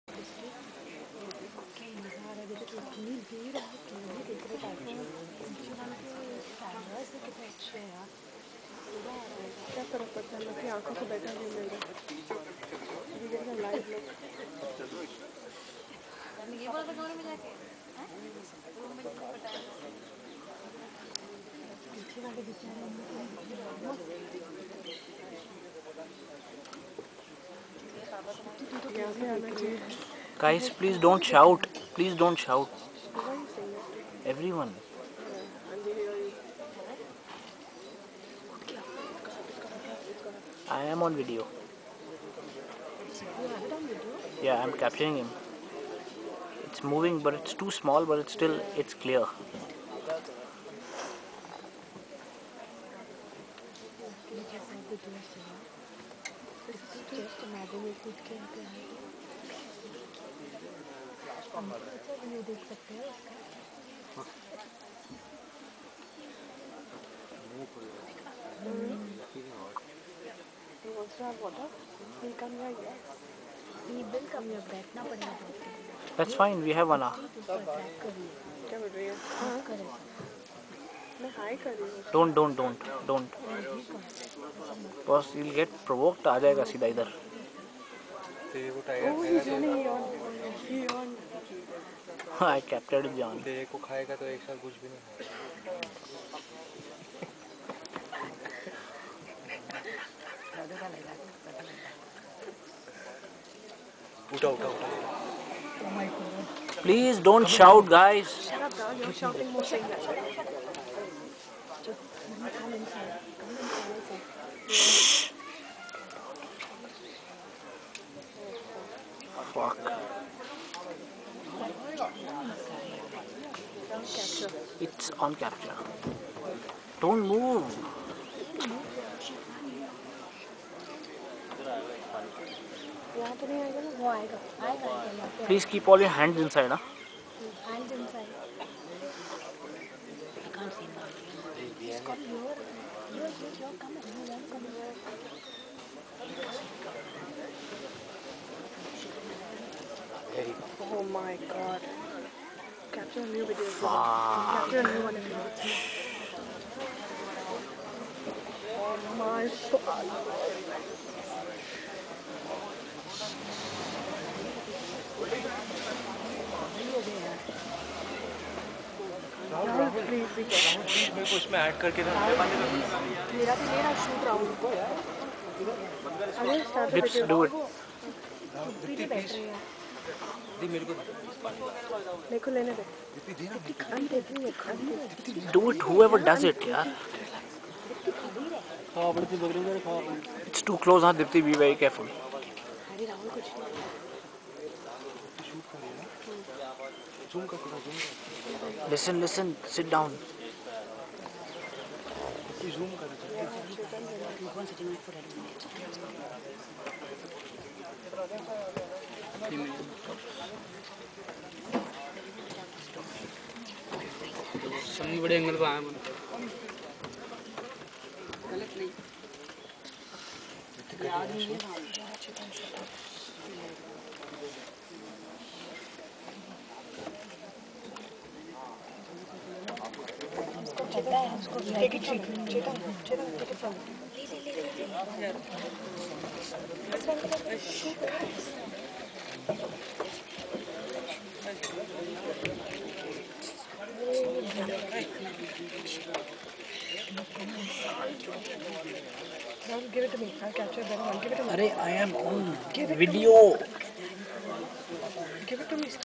Female Tiger at Ranthambhore Jungle.MPG
The most magnificent animal captured on video during one of our jungle safari's.